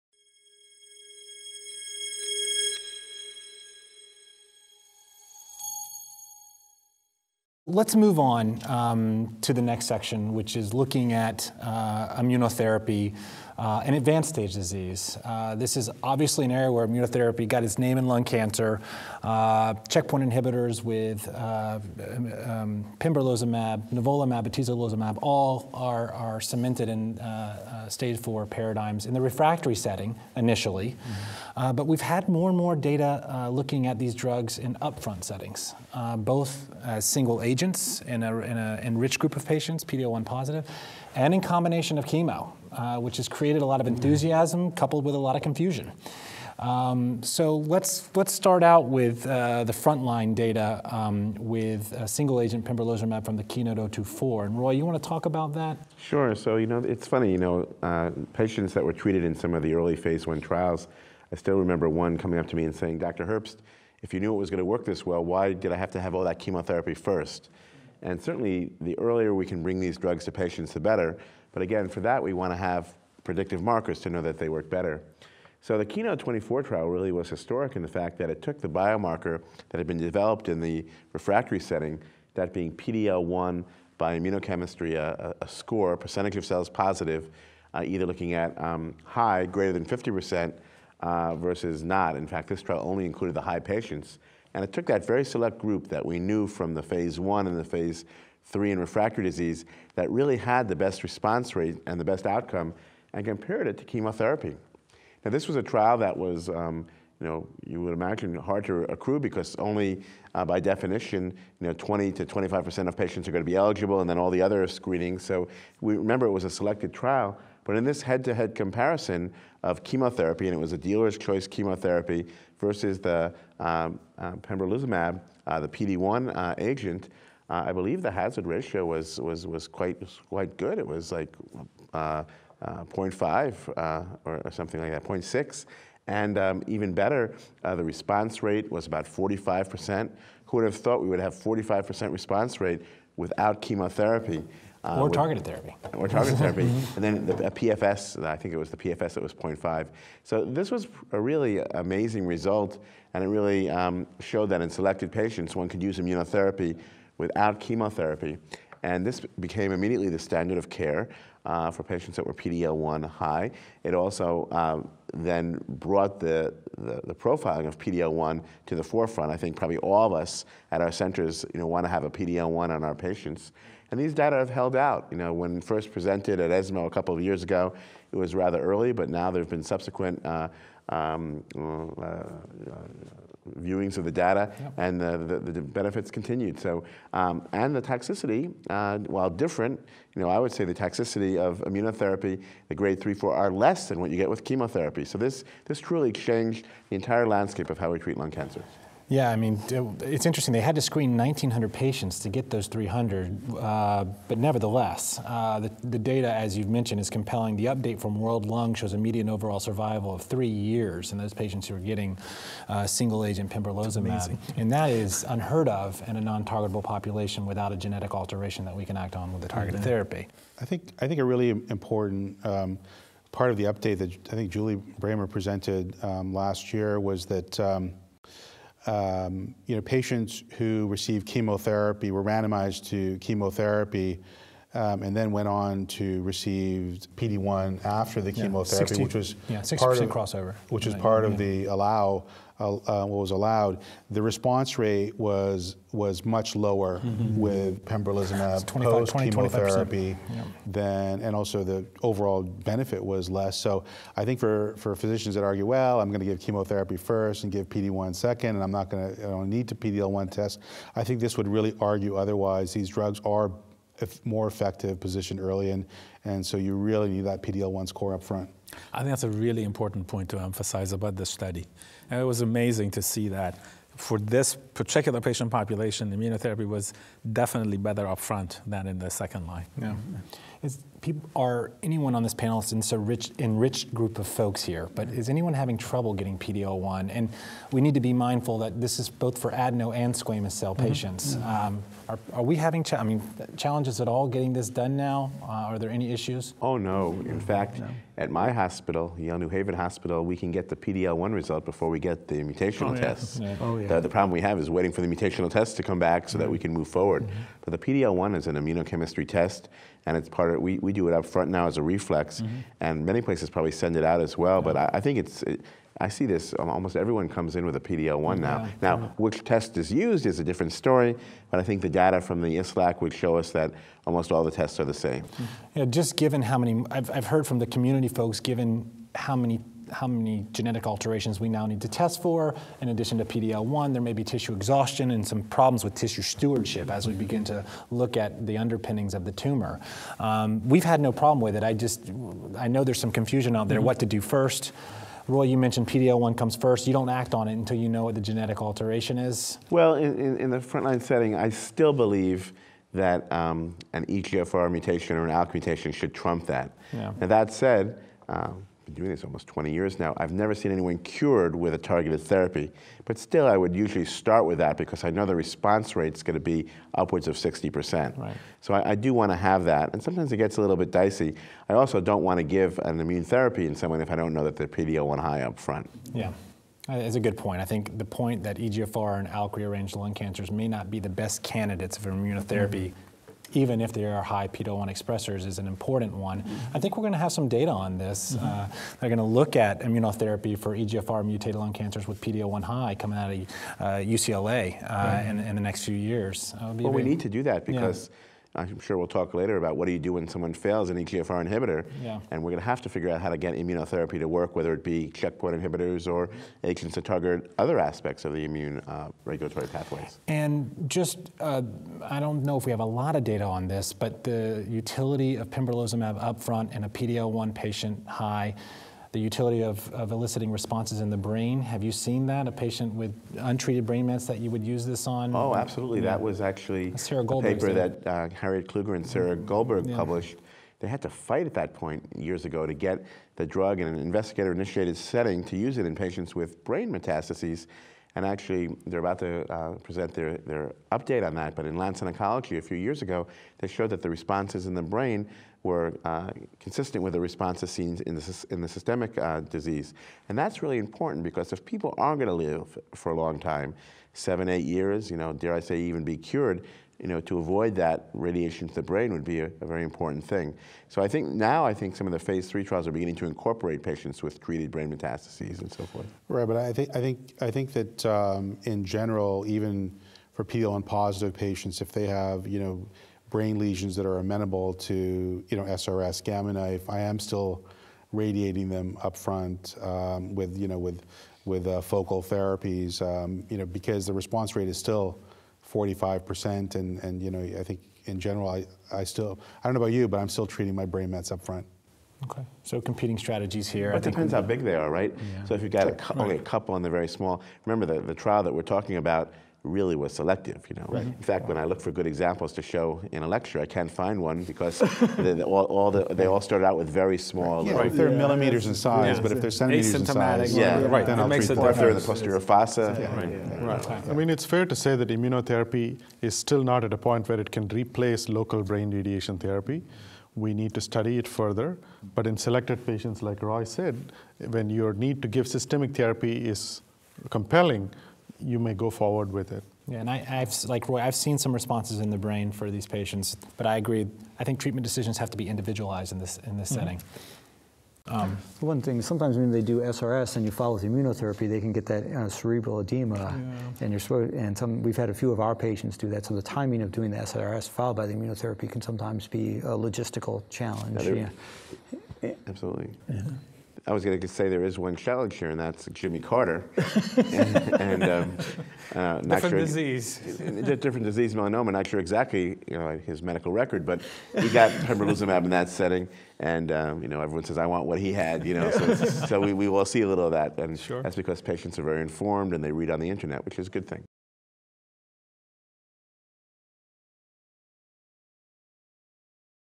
Checkpoint Inhibitors for Newly Diagnosed Stage 4 NSCLC
Benjamin P. Levy, MD; Roy S. Herbst, MD, PhD; Hossain Borghaei, DO, MS; Naiyer A. Rizvi, MD; Suresh S. Ramalingam, MD; and Thomas E. Stinchcombe, MD, discuss the paradigm-changing clinical trial that led to the approval of pembrolizumab in the up-front setting of non–small cell lung cancer and practical considerations for use of immuno-oncology–based regimens in newly diagnosed patients.